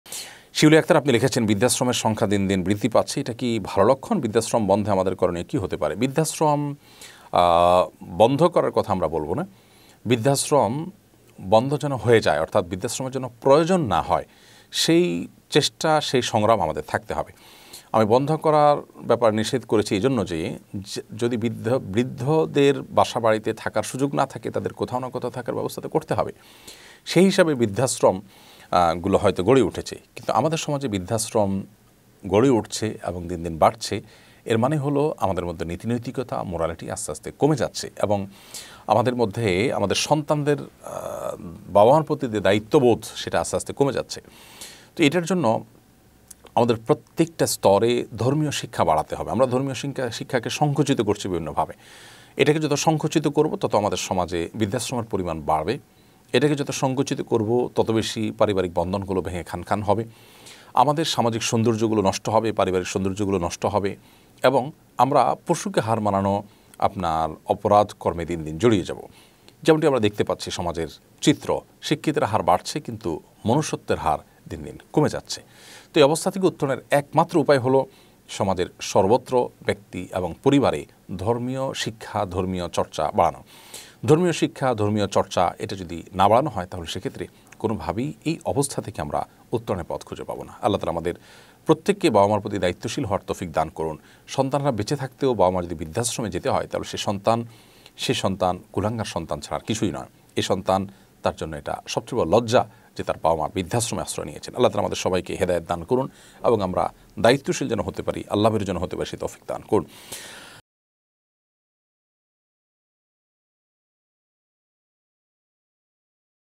বৃদ্ধাশ্রমের সংখ্যা দিন দিন বৃদ্ধি পাচ্ছে; মা বাবারা কী করবেন?
বৃদ্ধাশ্রমের সংখ্যা দিন দিন বৃদ্ধি পাচ্ছে; মা বাবারা কী করবেন? new waz II waj II Sheikh ahmadullah